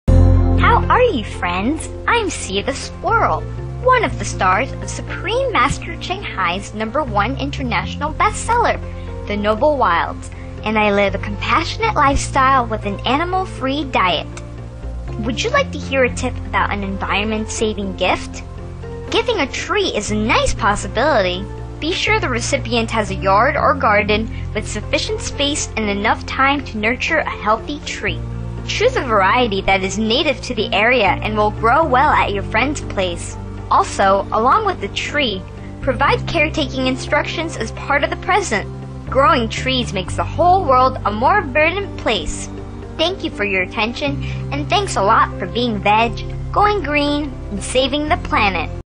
Environment-saving gift 贈送環保禮物
Sy the Squirrel: Giving a tree as a gift is a nice possibility. Be sure the recipient has sufficient space and enough time to nurture a healthy tree. Choose a variety that is native to the area. Also, along with the tree, provide caretaking instructions as part of the present. Growing trees makes the whole world a more verdant place.
賽(松鼠)：送樹給朋友是禮物的好選擇，要確定對方有足夠的空間和時間，來培與一棵健康的樹。選擇當地原產的樹種，將會在您朋友地方，長得很好。在送樹時，也一併送上種植指南。樹木生長，讓全世界更加青翠。(100614)